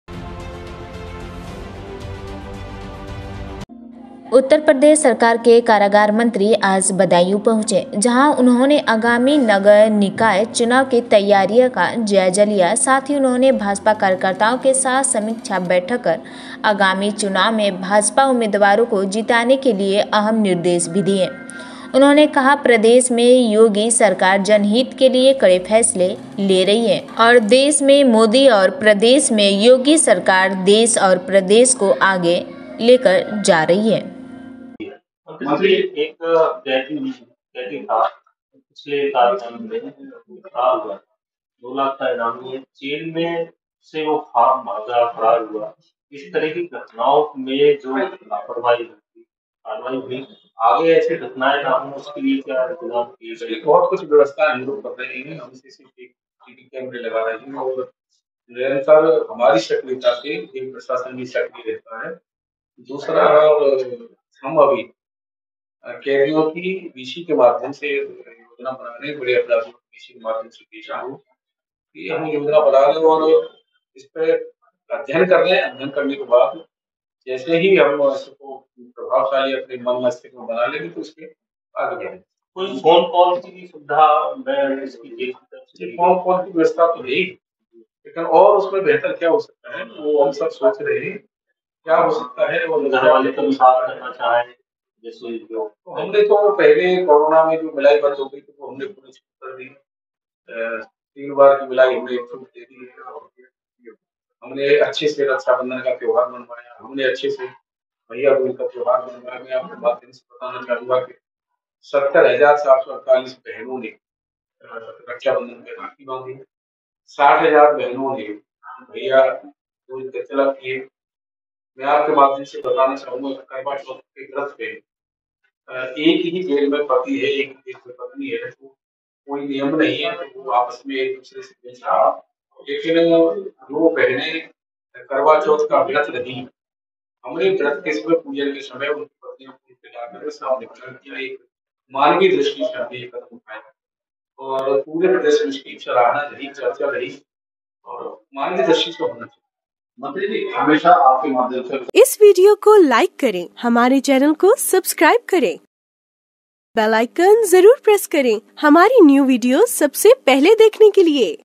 ||Badaun|| नगर निकाय चुनाव क़ा जायजा लेने पहुचें कारागार मन्त्री, योगी सरकार ले रही कड़े फैसले ?
||Shivam bhardwaj|| बदायूँ
उत्तर प्रदेश सरकार के कारागार मन्त्री आज बदायूं पहुचे।

जहाँ उन्होने आगामी नगर निकाय चुनाव की तैयारियां क़ा जायजा लिया साथ ही उन्होने भाजपा कार्यकर्ताओ के साथ समीक्षा बैठक कर आगामी चुनाव में भाजपा उम्मीदवारों को जिताने के लिए कई अहम निर्देश दिए।

उन्होने कहा प्रदेश में योगी सरकार जन हित के लिए कड़े फैसले ले रही है।

 और देश में मोदी और प्रदेश में योगी सरकार देश और प्रदेश को आगे लेकर जा रही है

आवश्यक सूचना -